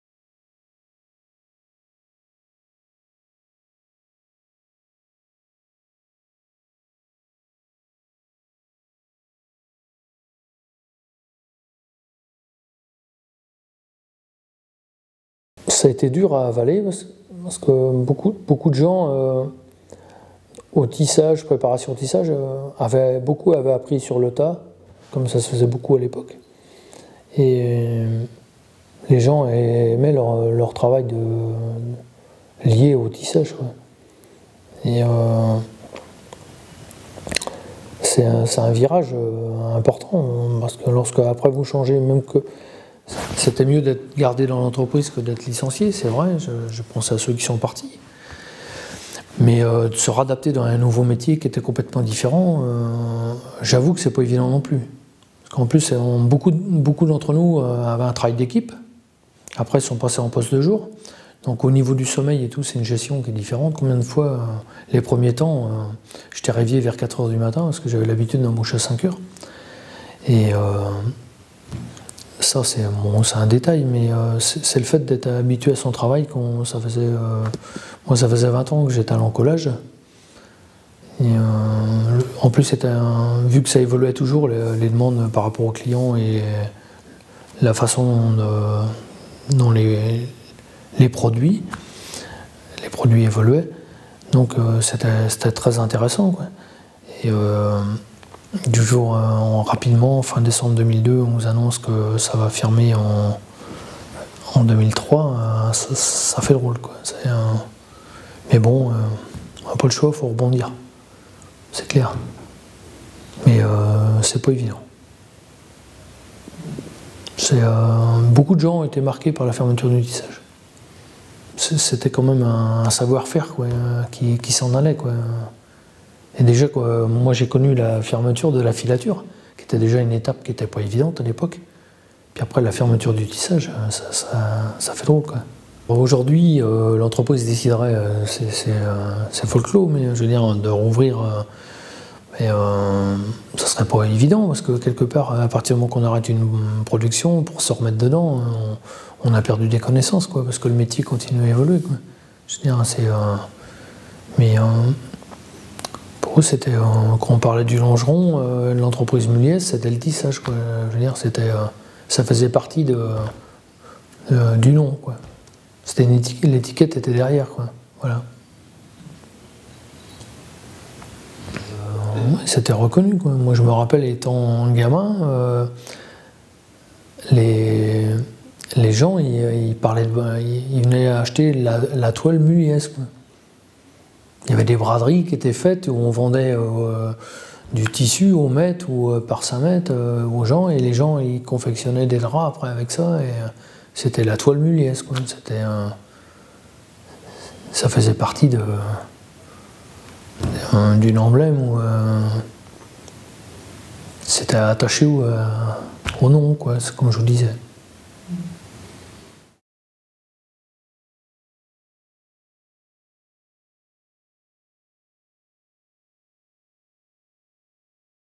Vidéo Moine et Sèvre: Regards industriels: " Perte de savoir et perte d'identité"
Témoignage recueilli dans le cadre de l'Inventaire sur la Communauté de Communes de Moine et Sèvre en lien avec la notice
 M.Philippe Rotureau, employé de l'usine Mulliez-Flory, parle de la perte de savoir et de la perte d'identité : la fermeture du tissage vécue comme une perte de savoir irréversible et comme une perte d'identité pour l'usine